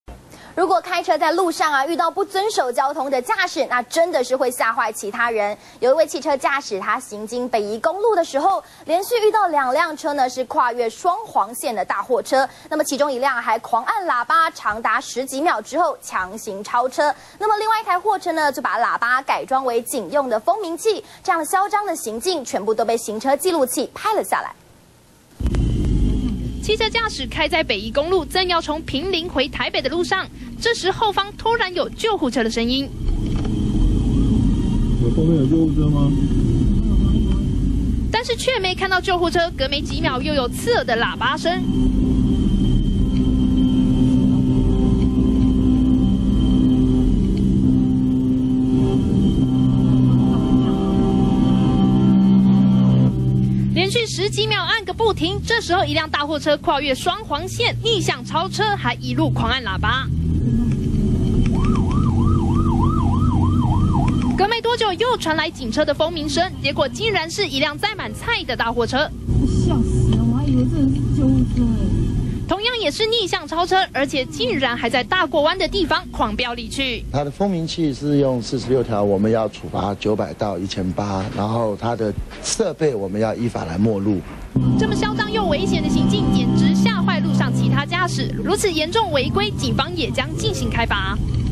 囂張貨車裝警笛 沿路逆向超車
中視新聞》一名駕駛行經北宜公路，恰巧碰到兩輛大貨車狂按喇叭疑似要超車，其中一台更誇張的把喇叭改成救護車的蜂鳴器，兩輛大貨車沿路按喇叭逆向飆車，行徑張狂，全被行車紀錄器給拍下來。